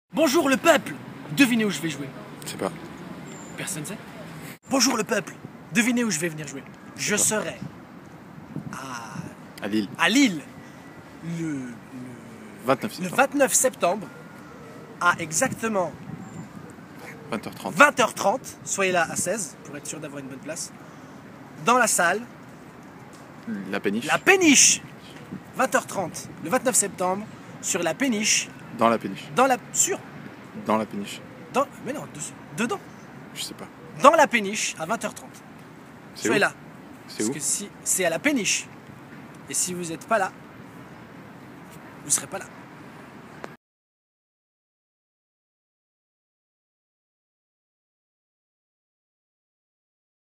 Mon producteur est ... un producteur, épisode 4: A La Péniche ou dans La Péniche?
Suivez le processus de création de campagnes de communication. Jouer dans une péniche qui s'appelle "La Péniche"... Mon producteur est bête